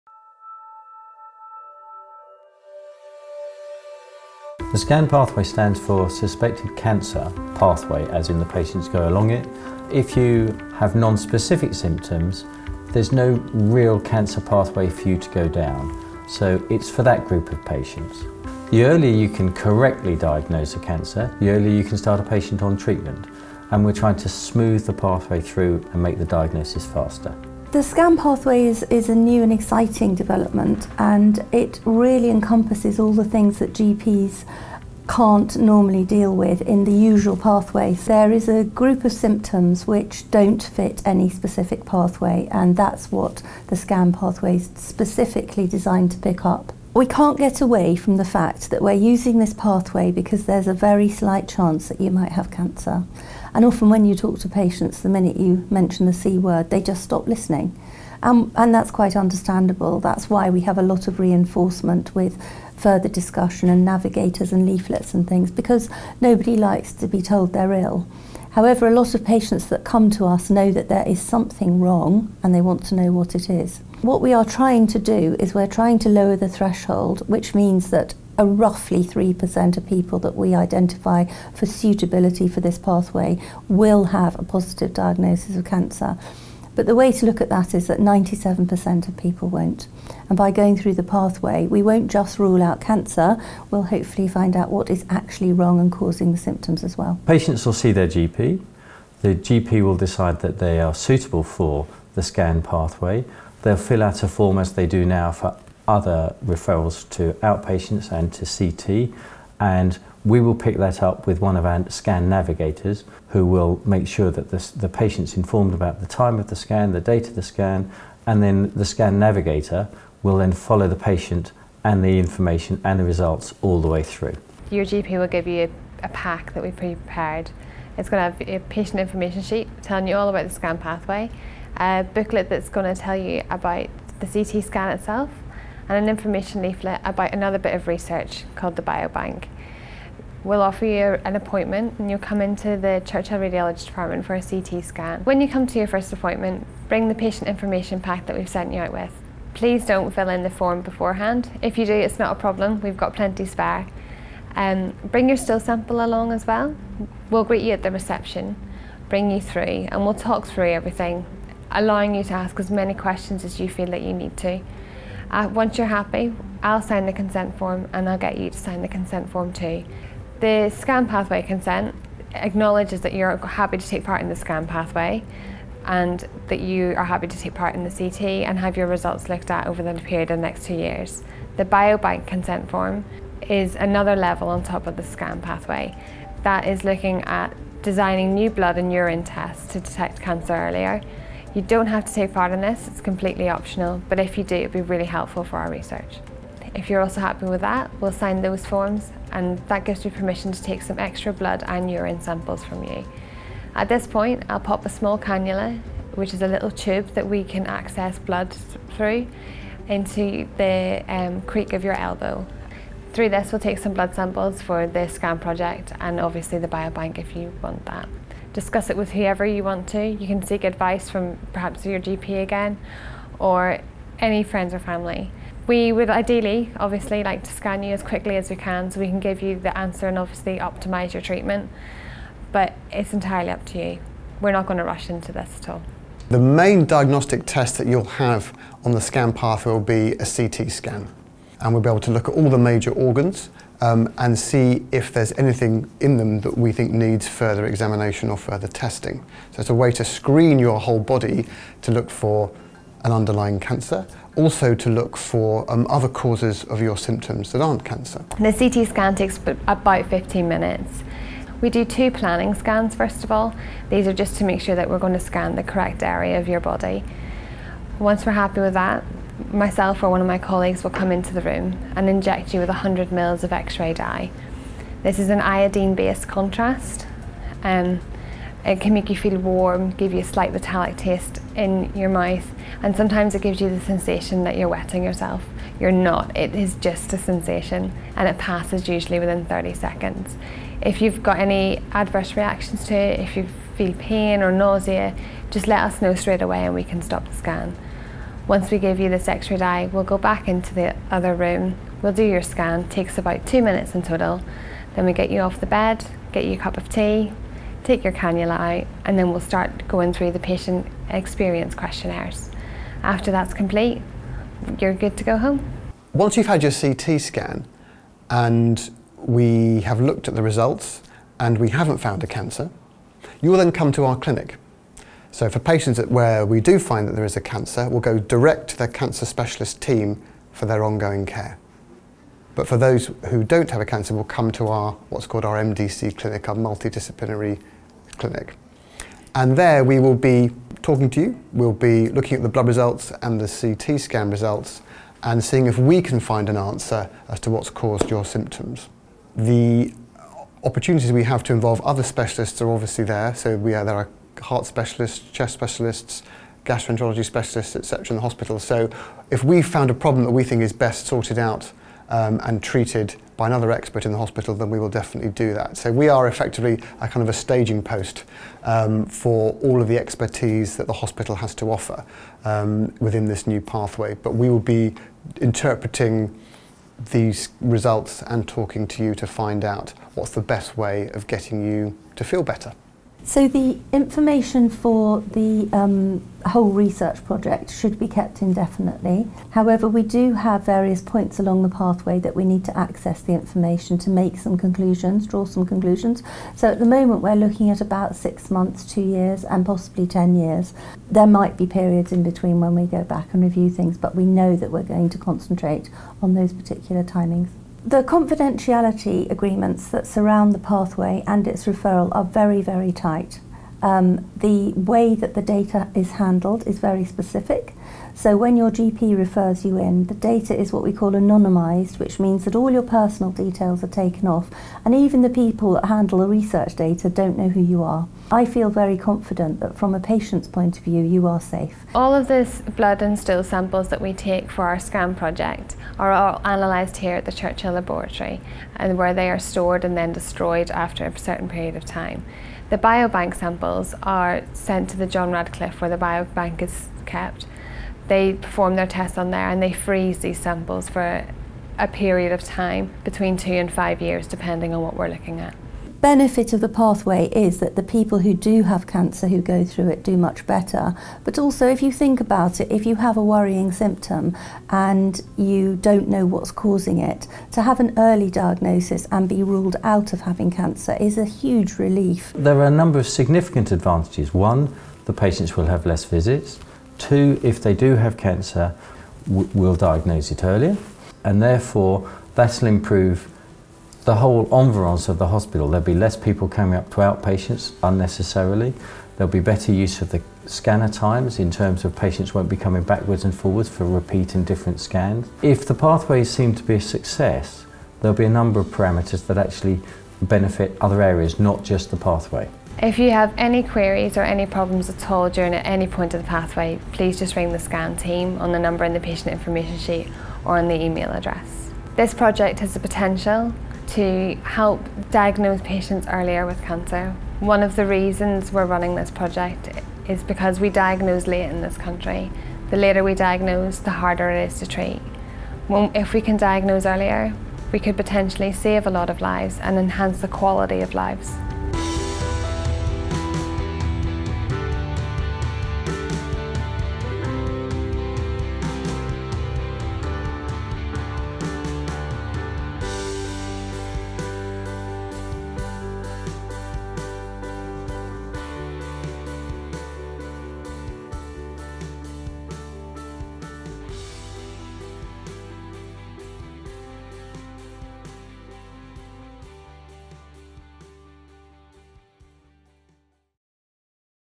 Suspected CANcer (SCAN) diagnostic pathway
"One of the reasons we are running this project is because we diagnose late in this country; the later we diagnose, the harder cancer is to treat.

If we can diagnose earlier we can potentially save a lot of lives and enhance the quality of lives."

The Suspected CANcer (SCAN) diagnostic pathway is a pioneering collaborative project supported by Oxfordshire Clinical Commissioning Group, Cancer Research UK and the University of Oxford Nuffield Department of Primary Care Health Sciences.

The project is being run by Oxford University Hospitals NHS Foundation Trust.

The project has the potential to save lives and improve cancer outcomes for patients with early non-specific symptoms by reducing the time to diagnosis.